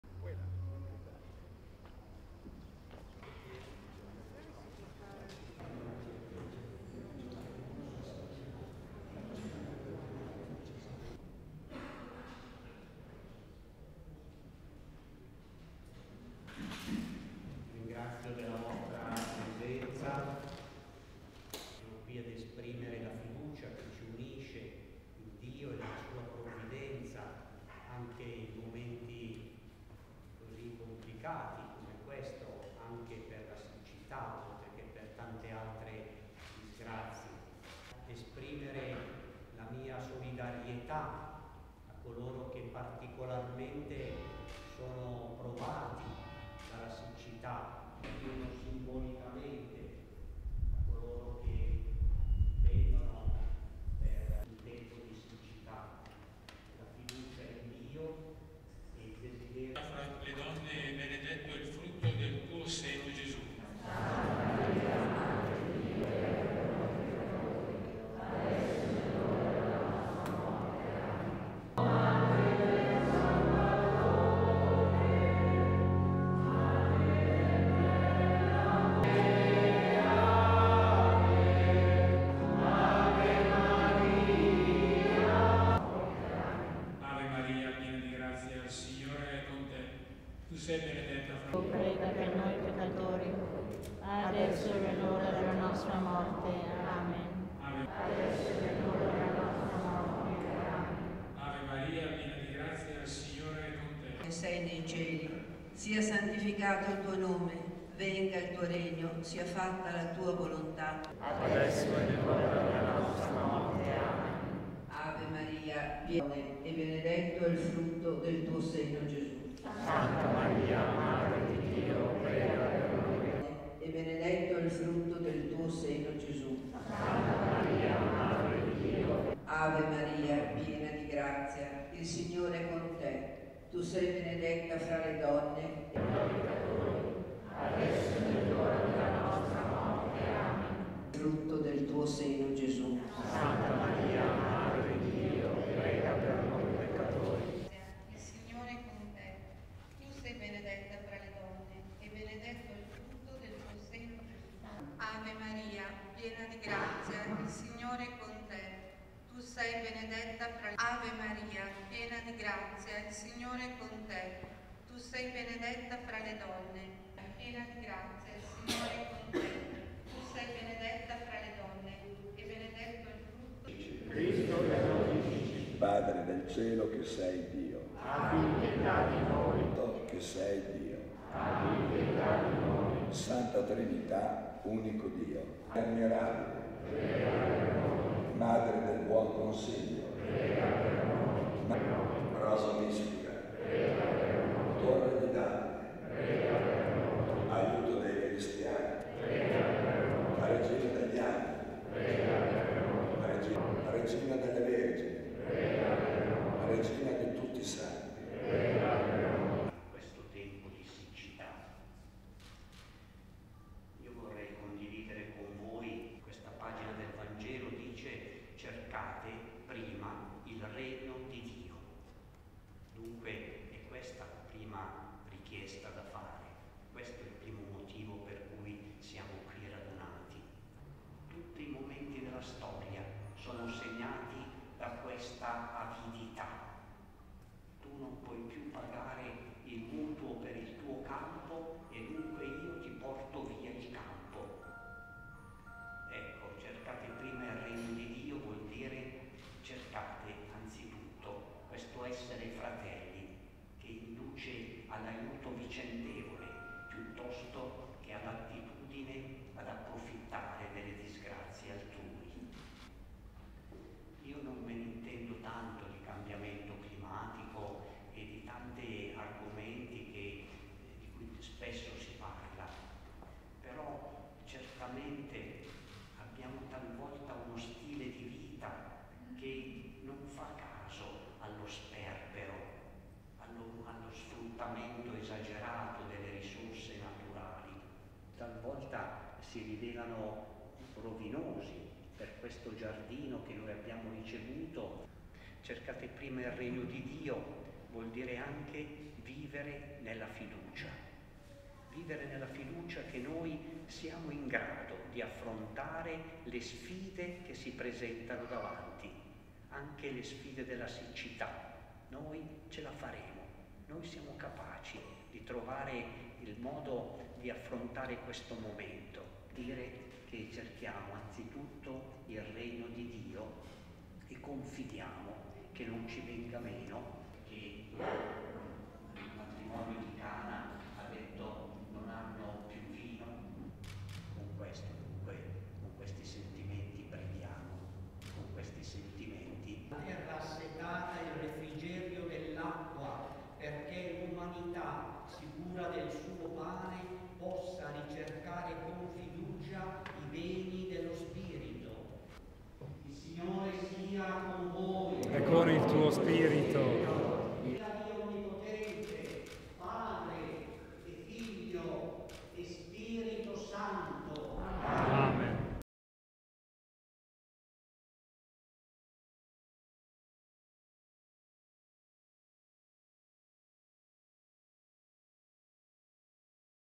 Siccità, si prega per la pioggia: monsignor Delpini a Trezzano S/N
L’arcivescovo di Milano, monsignor Mario Delpini, ha intrapreso quello che lui stesso ha definito un “pellegrinaggio in tempo di siccità”. Un momento di preghiera collettiva in alcuni comuni agricoli duramente colpiti dalla grave crisi idrica che sta mettendo in ginocchio sopratutto l’agricoltura. Nelle immagini la prima delle tre tappe previste, quella a Trezzano sul Naviglio.